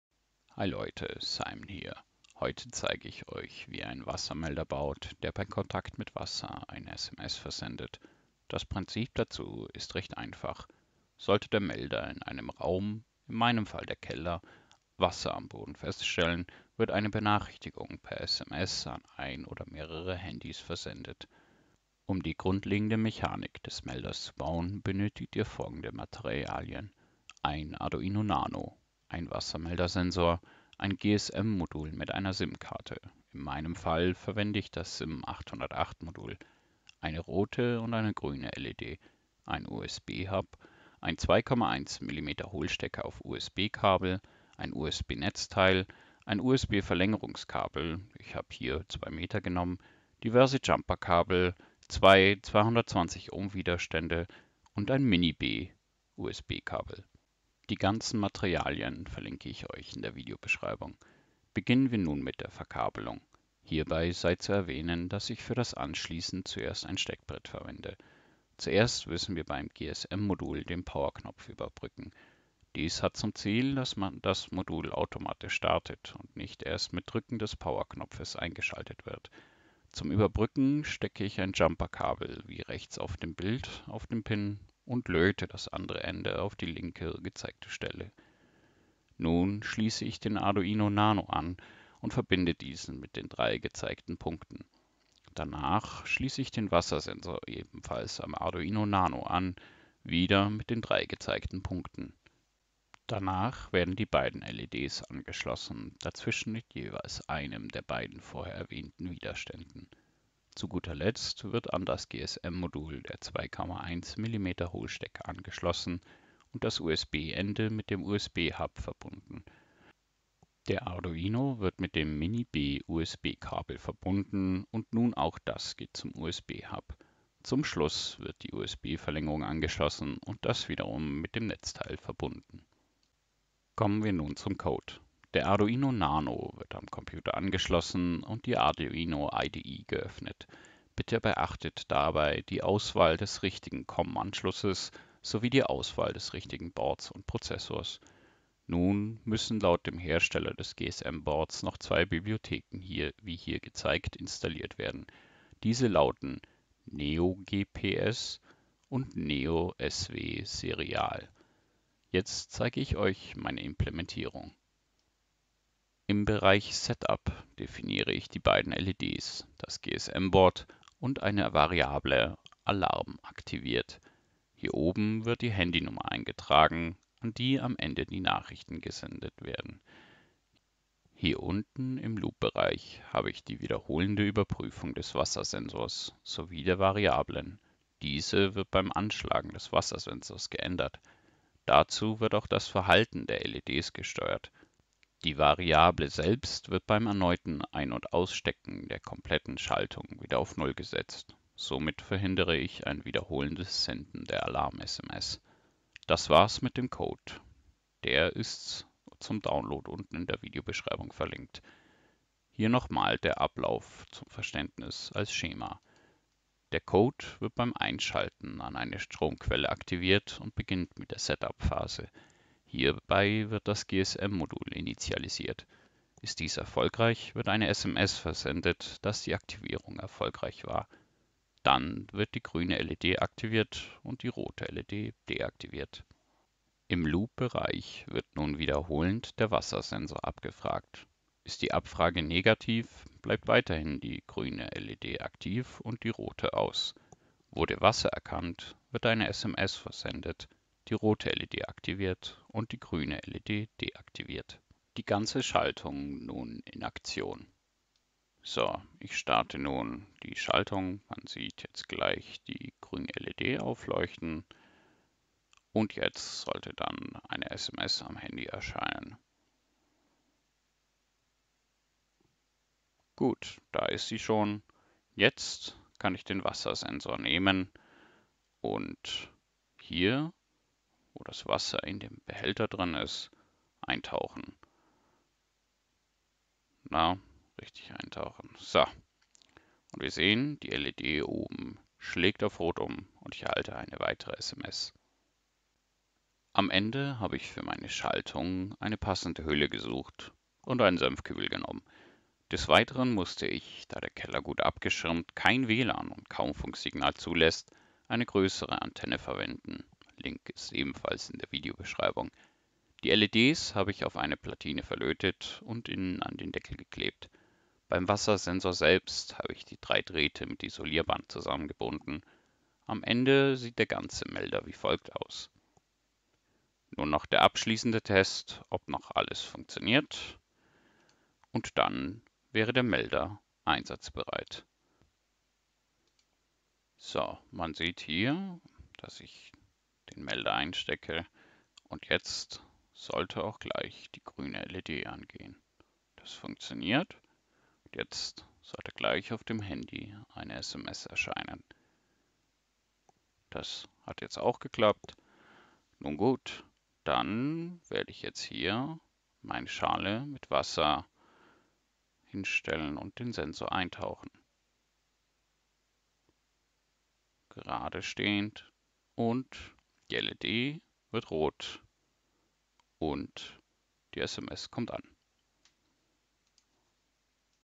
Wassermelder per SMS mit Arduino Nano und SIM 808 (Engl. Sub)
0:00 Intro
0:10 Funktion
0:27 Materialien
1:03 Verkabelung
2:13 Code
3:36 Schema
4:18 Test
4:56 Finaler Zusammenbau
5:36 Finaler Test

Links zu den Materialien:
Arduino Nano